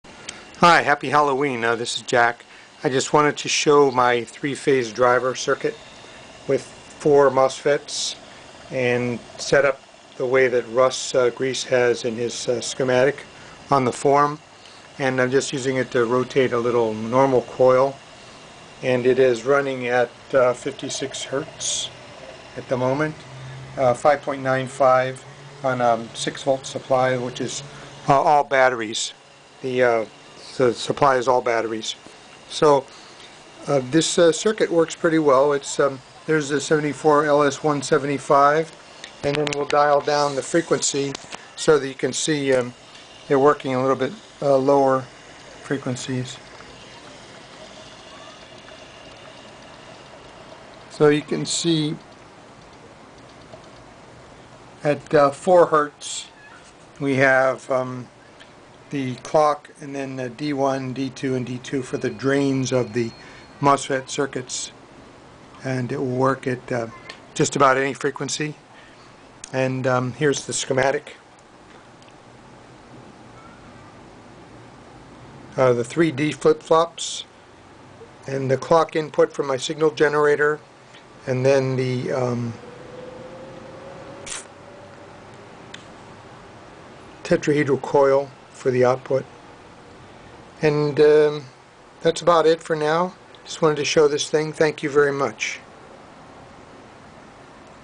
3 Phase Driver
3 Phase Mosfet driver for energizing coils.  Uses a 74LS175  D-flip-flop IC to fire the gates of the mosfet transistors in sequence.    Provides one circui for the Clock CLK, and D1, D2, and D3.    All of the outputs may be connected to one end f a coil winding to act as a high-speed switch.    The D1, D2, D3 outputs ar 120 degrees out of phase.      The input CLK is provided by a signal generator.      This video only shows D3 hooked up for a preliminary test.   Later I'll hookup all 4 coils of my tetrahedron coil.       Schematic is included in video.  Note, the LEDs and associated resistors are for testing only, and should be removed after the circuit is working.   Back-spike voltages blow them out frequently.    Note on Q4,  I put a diode between the Drain and +6V  this lets the LED live longer,  and directs the back-spike voltage from the coil to the +6V volt, charging it up a bit  perhaps.          Big thanks to Russ G.

visit the new vortexspace.org site which is great info
aloha